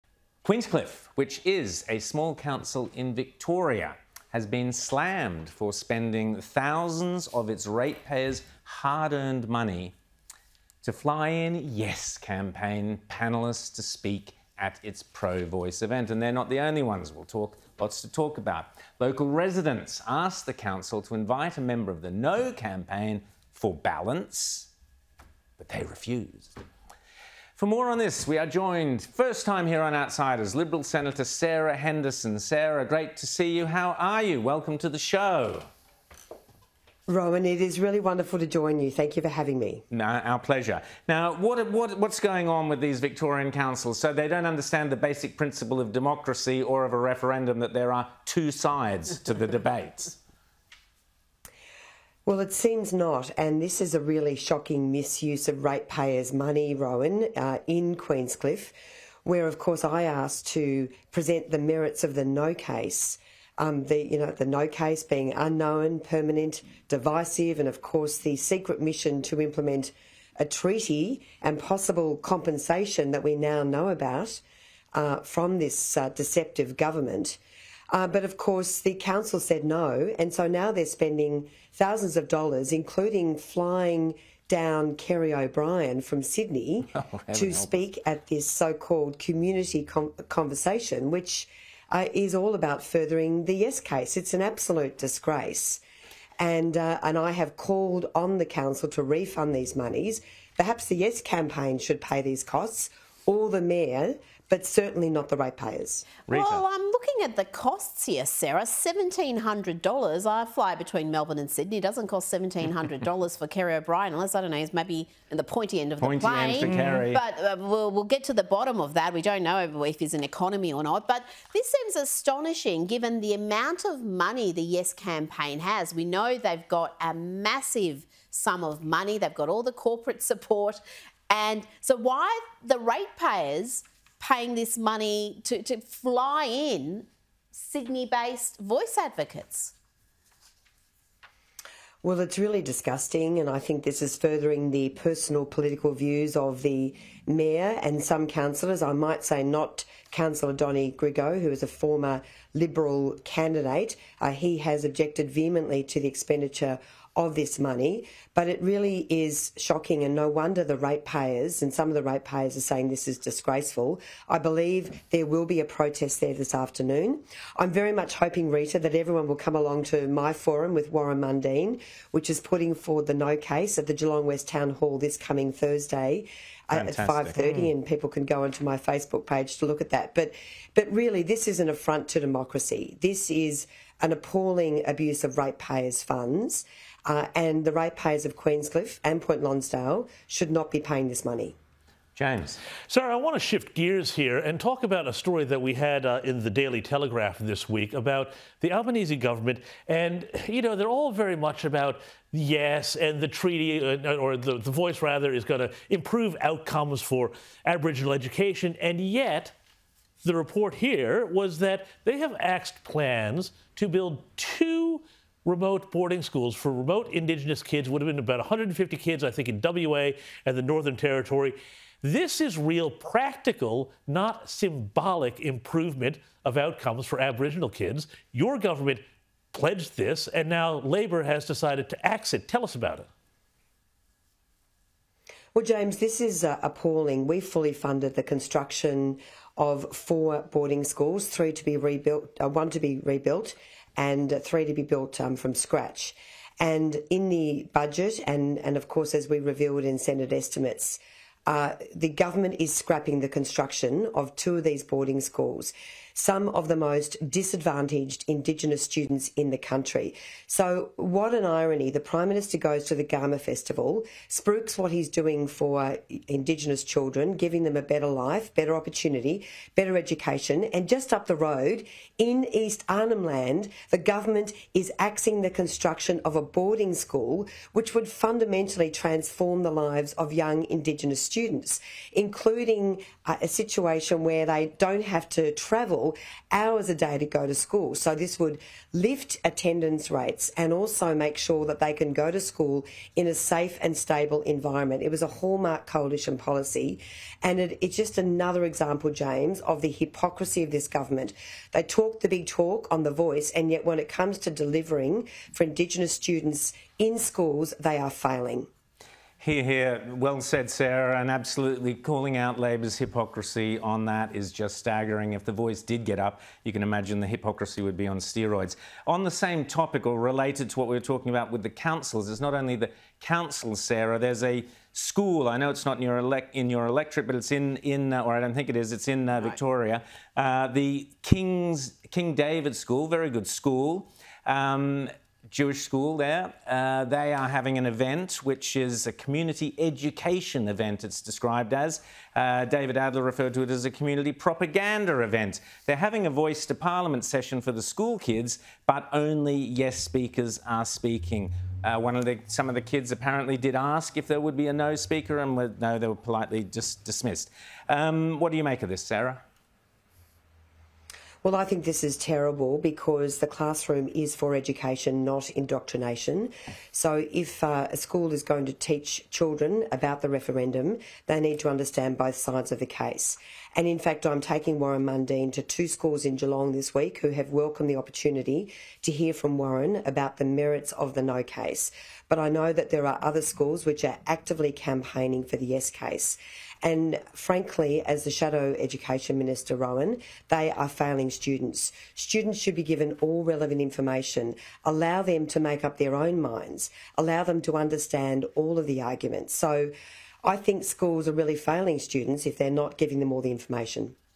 Classrooms are for ‘education not indoctrination’: Liberal Senator
Liberal Senator Sarah Henderson has criticised schools for advertising the ‘Yes’ campaign saying classrooms are for “education, not indoctrination”.

“If a school is going to teach children about the referendum, they need to understand both sides of the case,” Ms Henderson told Sky News Australia.

“There are other schools which are actively campaigning for the yes case … they are failing students.

“Students should be given all relevant information, allow them to make up their own minds, allow them to understand all of the arguments.

“I think schools are really failing students if they are not giving them all the information.”